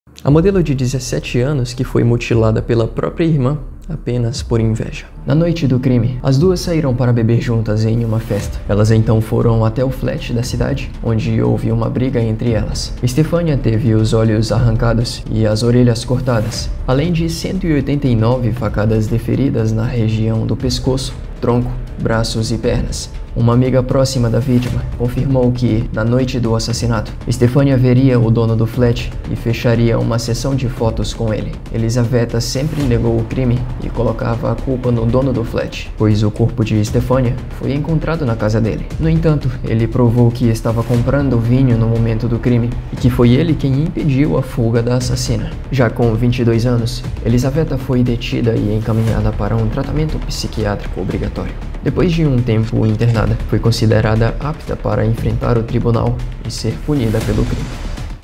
O TERRÍVEL ASSASSINATO DE STEFANIA DUBROVINA
Conheça o terrível caso do assassinato de Stefania Dubrovina, a modelo de 17 anos que foi brutalmente assassinada pela própria irmã.

Siga no Instagram @lugarsilencioso e o perfil pessoal @lipfont
Siga a página oficial do canal no Instagram: @lugarsilencioso